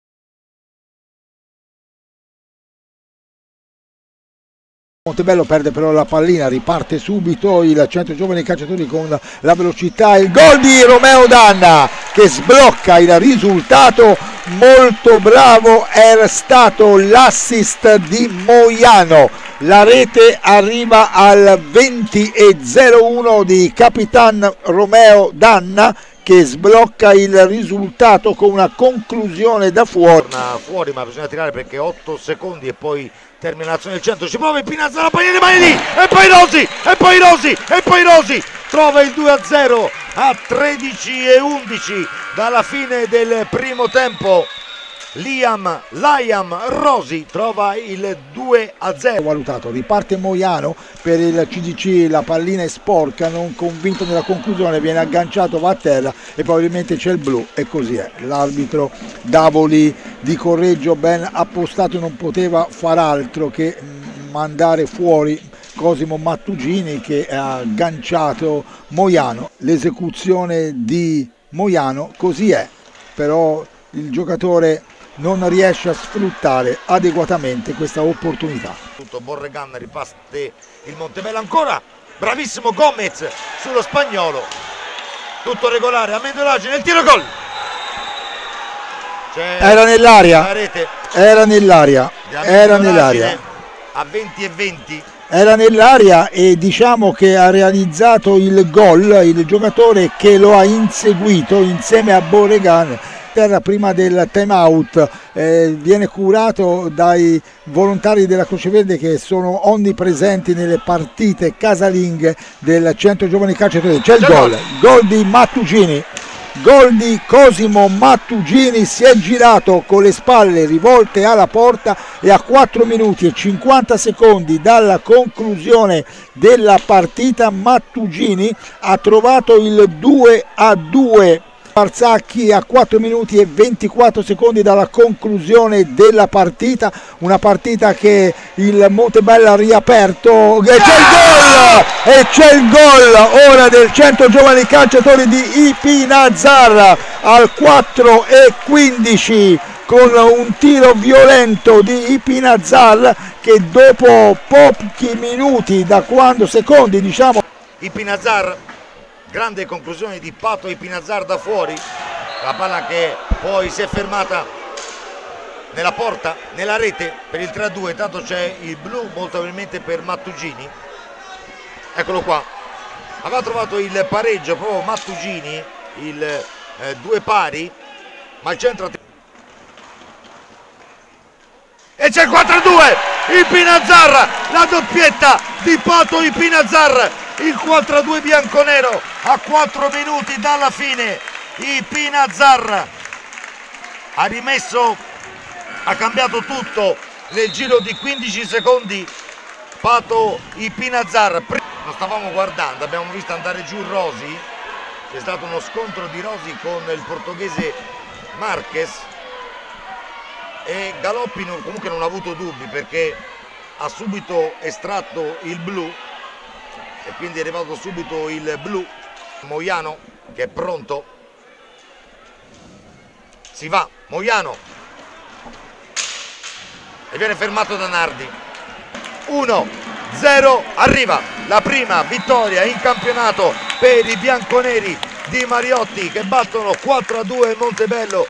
Highlights - Giornata n.7 - C.G.C. Viareggio x Tierre Chimica Montebello H.P.
hockeypista hpseriea1 - Campionato Serie A1 - Regular Season - Giornata n.7 - C.G.C. Viareggio x Tierre Chimica Montebello H.P. - PalaBarsacchi di Viareggio (LU) - Mercoledì 23 novembre 2022